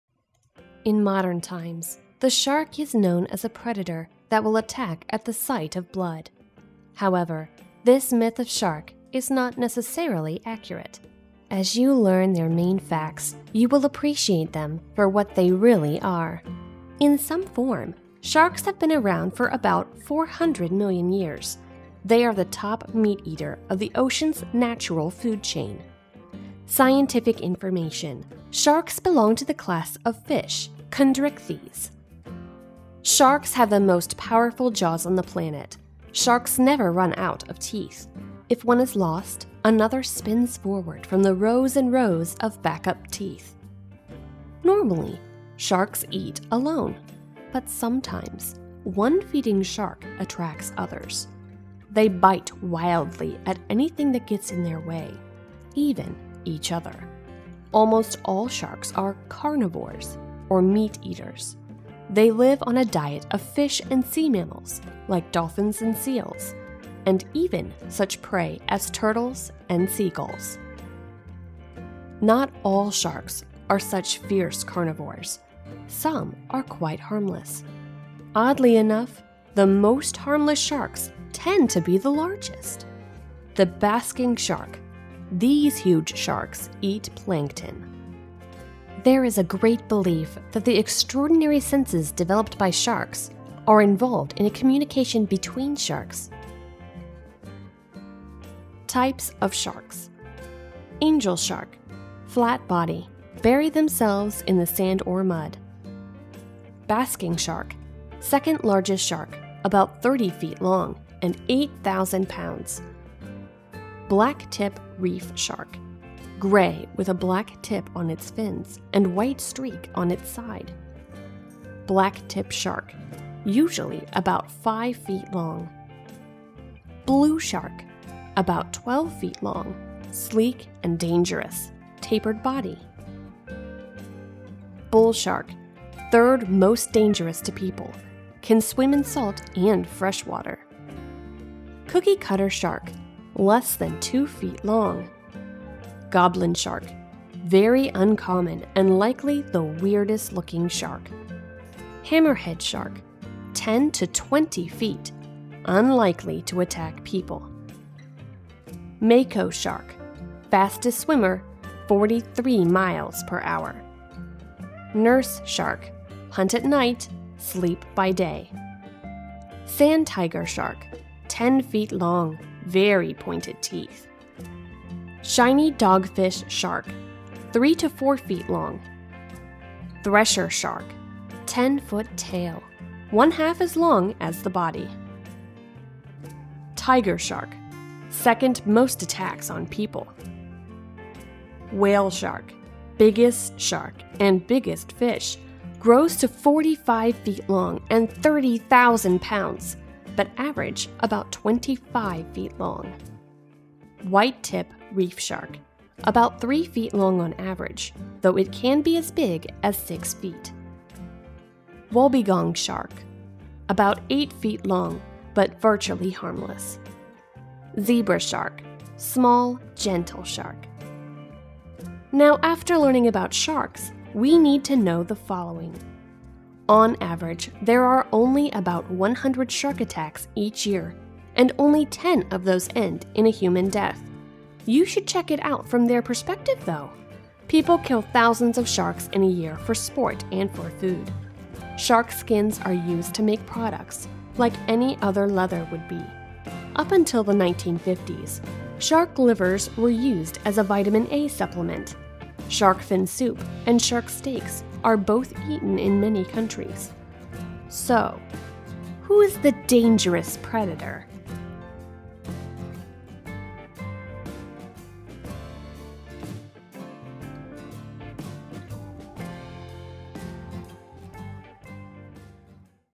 Learn about Sharks | Types and Habitat | Sharks video
Sharks give birth to their young ones just like other mammals, but they are species of fish and not mammal.

Let's explore the amazing world of SHARKS with this video.